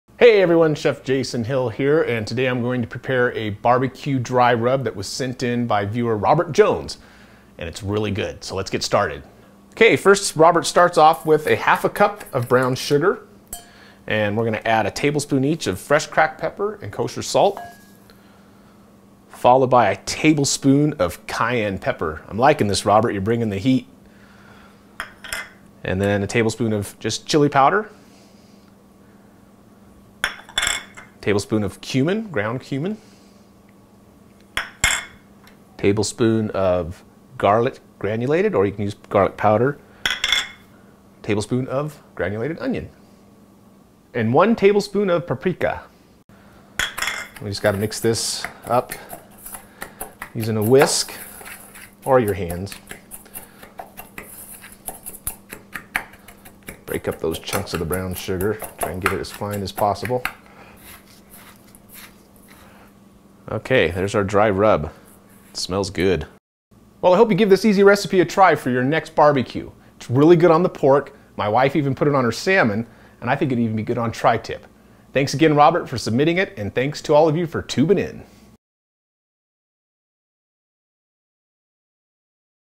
BBQ Dry Rub Recipe
Chef Jason Hill shares this BBQ dry rub recipe from viewer Robert Jones. This sweet and spicy recipe for barbecue dry rub is delicious served on pulled pork, smoked salmon and trip tip. It's incredible!